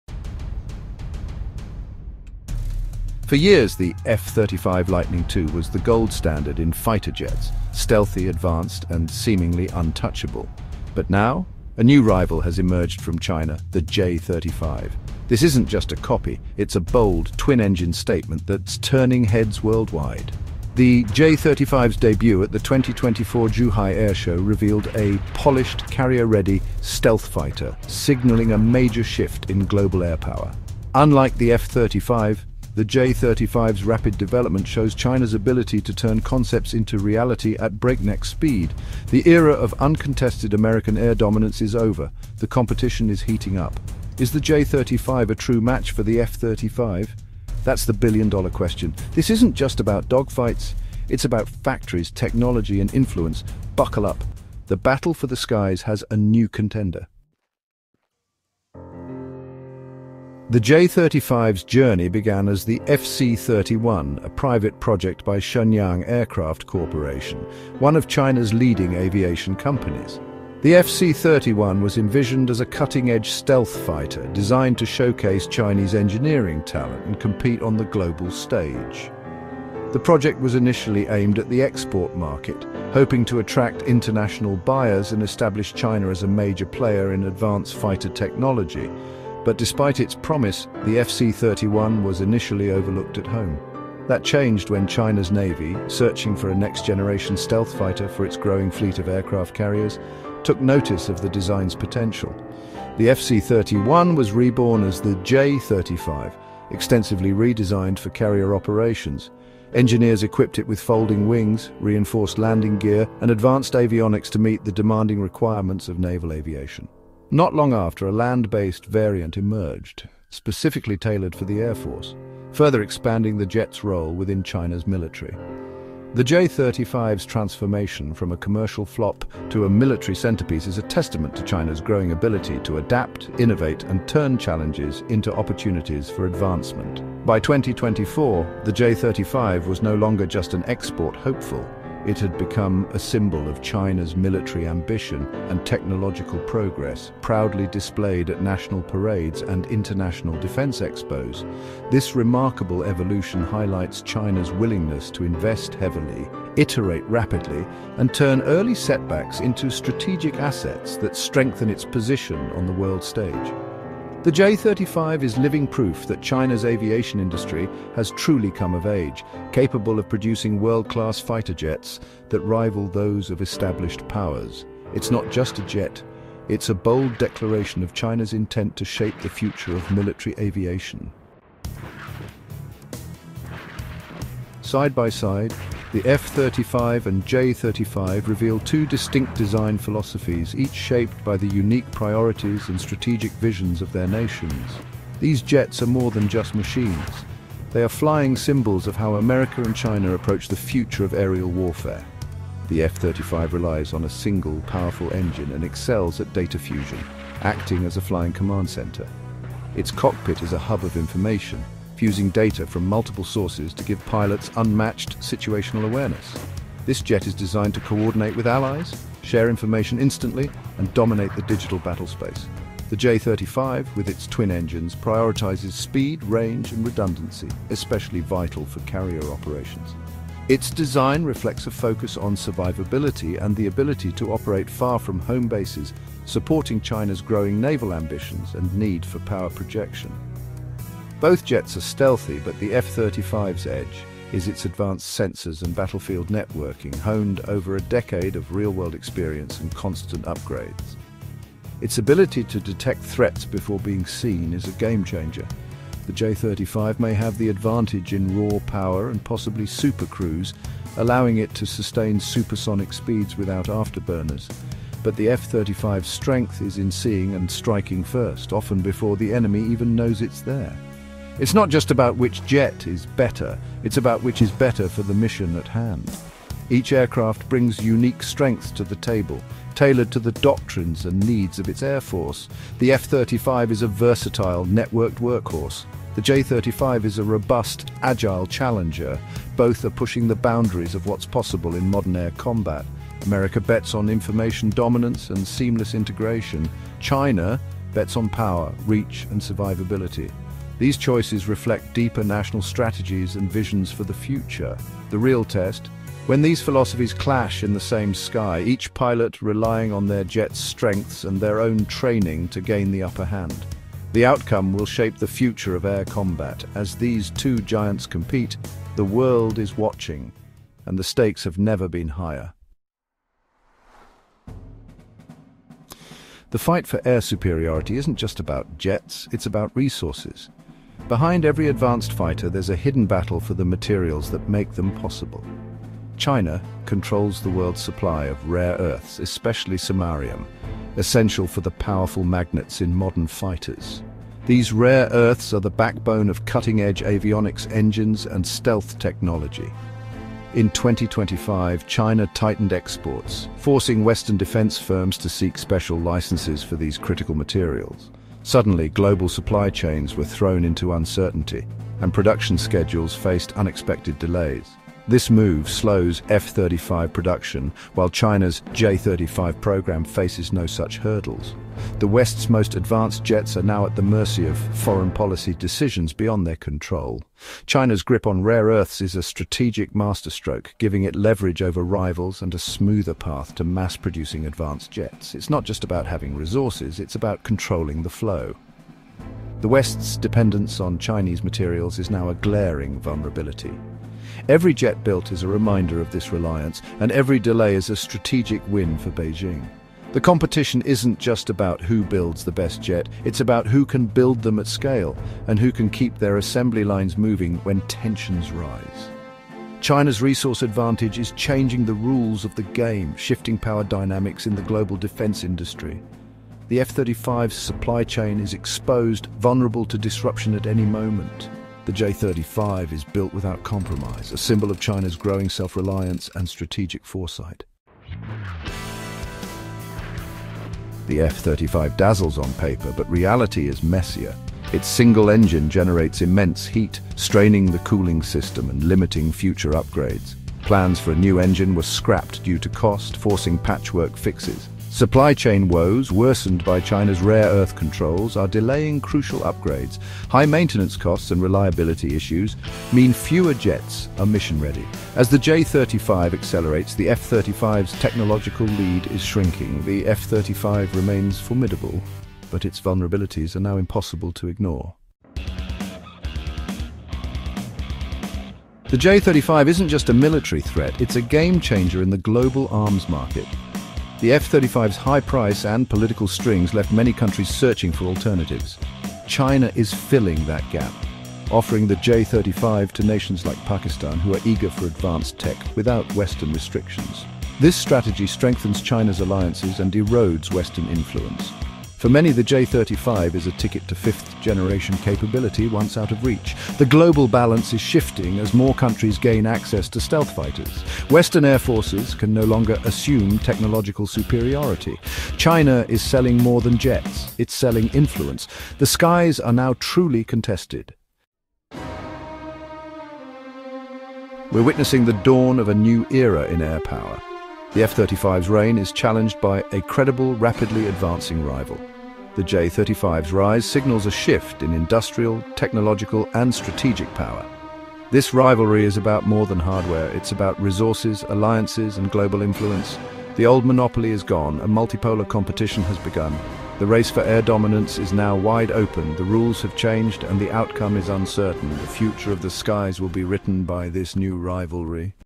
How China’s New Fighter Jet Outclasses the F-35
🇨🇳 How did China’s latest fighter jet leap ahead of the U.S. F-35? In this video, we take an inside look at China’s next-generation stealth aircraft — examining its cutting-edge technology, unmatched speed, and advanced weaponry. From radar-evading design to superior dogfighting capabilities, discover how this jet could change the balance of air power. Are U.S. forces prepared for what’s coming?

Join us as we explore the implications of this development for global defense strategies.

00:00:00
A New Challenger Enters the Ring

00:00:57
The J-35's Story

00:03:01
Two Jets, Two Philosophies

00:05:22
China's Ace in the Hole

00:07:07
The F-35's Troubles

00:07:45
A New Game in the Global Skies

00:08:31
The Dawn of a New Air Power Era